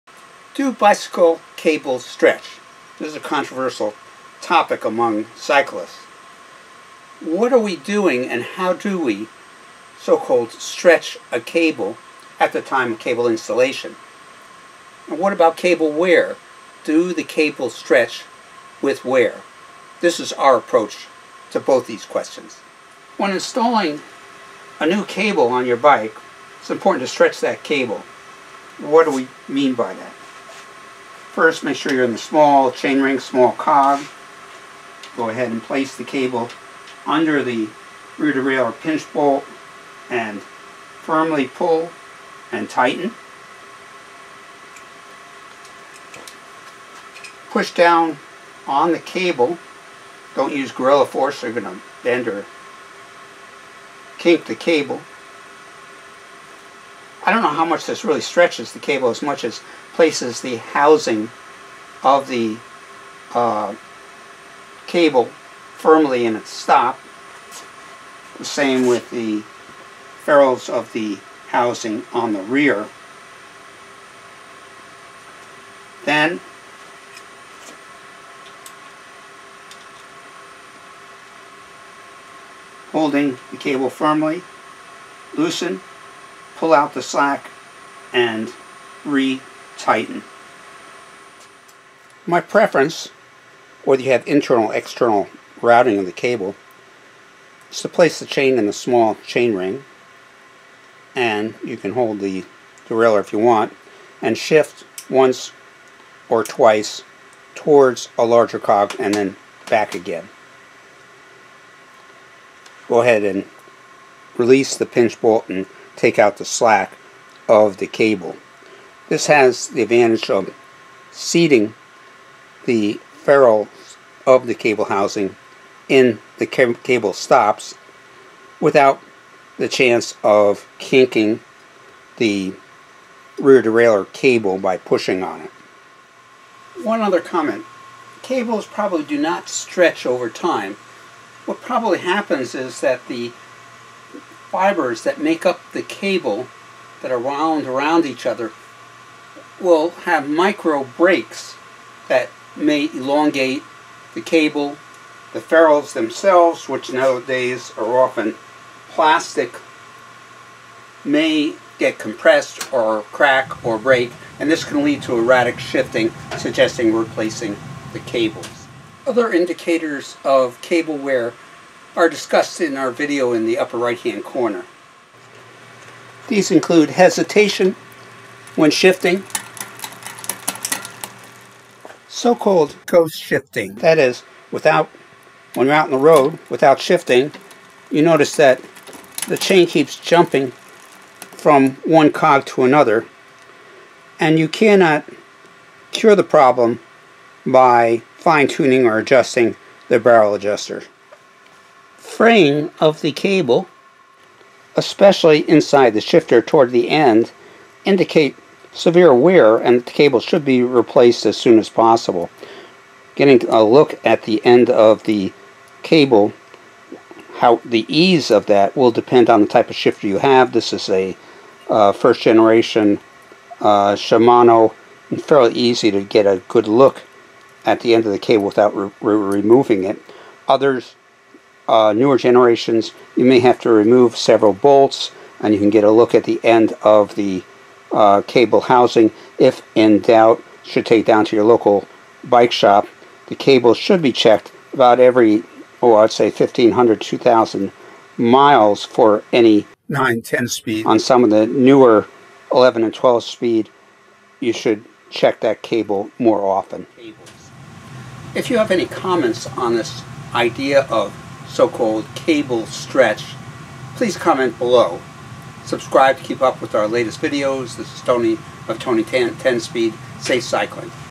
Do Bike Cables Stretch: Fact or Myth - stretching & wear
Bike Cable Stretch: Fact or Myth
Do bicycle cables really stretch? What are we doing when "stretching" cables at the time of installation. Doe the cables stretch with wear/
00:22 Cable Installation: We show you how to "stretch" your bicycle derailleur cables during new cable installation whether it be SRAM, Shimano, Campagnolo or other drive trains on a road bike or mtb using 2 different techniques for gear setup.
  00:45  Method 1 Place the bike chain in the small chainring and smallest cog. Attach the cable under the cable pinch bolt on the rear derailleur, pull out the slack of the gear cable and tighten the bolt. With externally routed cables, one can push down on the cable that runs along the down tube. Push firmly but not with gorilla force or you may kink the cable. Now release the pinch bolt and take out the cable slack before re-tightening the pinch bolt. *This probably doesn't really "stretch" the cable (part of the "myth"), but rather firmly pulls the cable housing and the housing ferrule into the cable housing stops.* Finally, loosen the rear derailleur pinch bolt and take out any remaining slack out of the derailleur cable before re-tightening the cable pinch bolt thus increasing the cable tension.
  01:32  Method 2 Preferred method: While in the small chainring and smallest cog (highest gear), remove any cable slack as above. Now, without turning the crank, shift toward the next one or two cogs and then back again. Again, take out any remain slack out of the cable by releasing the rear derailleur mech pinch bolt, firmly pulling on the cable (don't use gorilla force) and re-tighten the pinch bolt. This has the advantage of seating the derailleur housing ferrules in the stop whether you have internal cable routing or external cable routing and decreases the chances of kinking the cable.
This make indexing adjustment easier and quicker.

02:16 Cable Wear and Replacement: *With use, the cables probably don't really stretch (more of the "myth") but rather there may be micro breaks in the fiber weaving of the cables leading to lengthening or the ferrules (which these days are usually plastic) may become compressed, cracked or break - causing to eratic shifting and necessitating cable replacement which should improve shifting performance.* So when should you replace derailleur cables?
  1. Fraying, ESPECIALLY AT THE SHIFTER, at least every 1500 miles. How often you replace the derailleur cables depends on whether you have a 10, 11 or 12 speed. The higher speed have thinner chains that wear faster. Your local shop can give you advice in this area.
  2. Hesitation that cannot be eliminated by adjusting the cables with the rear derailleur barrel adjuster.
  3. Sometimes you can predict that the cables need changing when shifting feels soft or the chain starts jumping between cassette cogs without shifting despite adjustments of the barrel adjusters or adjustments on your work stand. For me, I know my cables will usually last 2000 miles and will change both front and back derailleur cables; earlier if I note fraying or shifting problems described above (10 speed, second generation Shimano). Better to be safe than breaking a cable on a steep uphill (which can spell disaster).